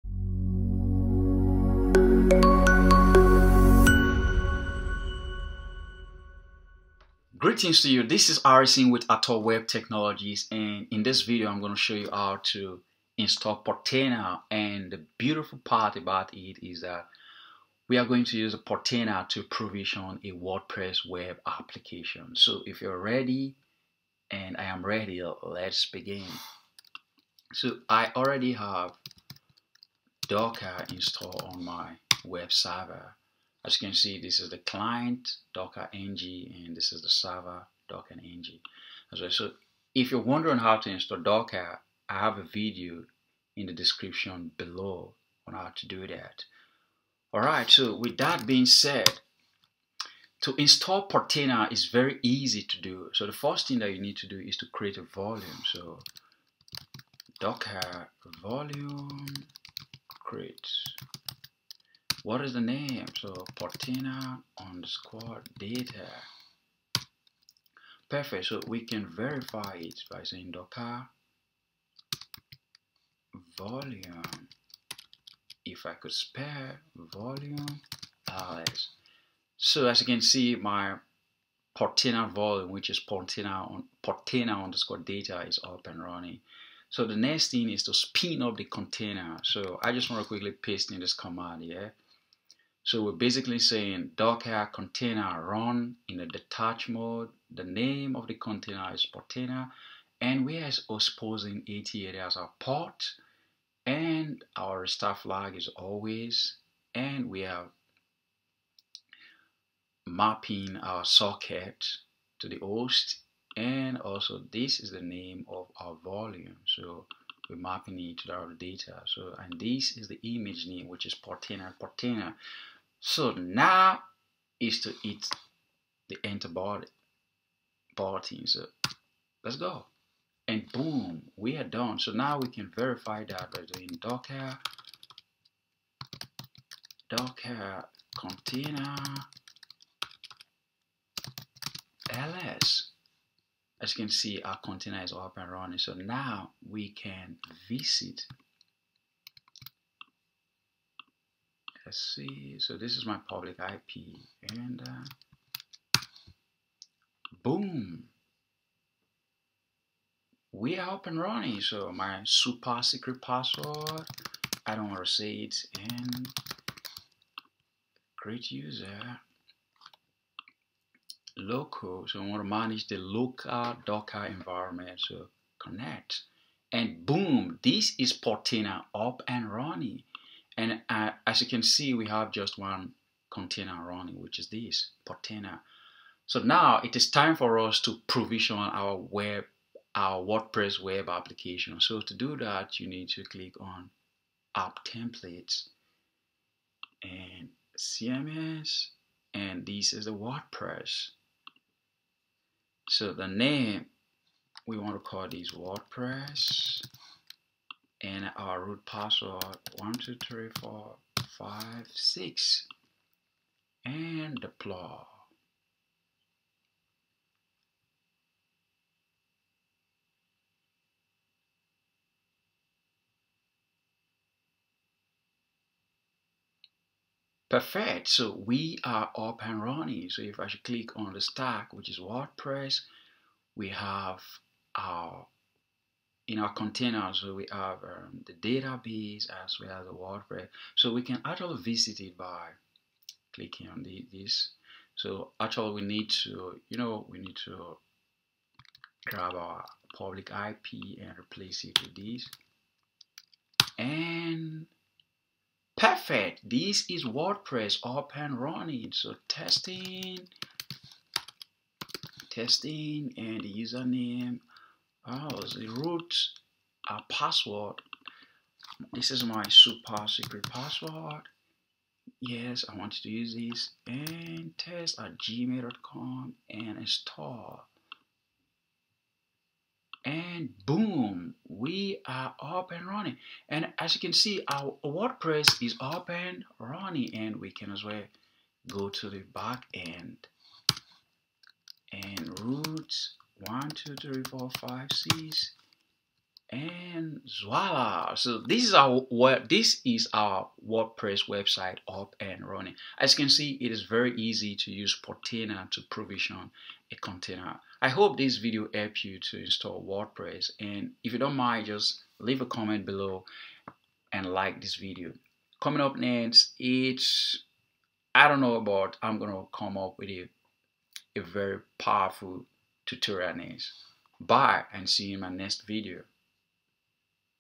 How to Install Portainer (Docker) | WordPress Installation using Portainer
In this video, I explain how to install Portainer in Docker & WordPress Installation using Portainer.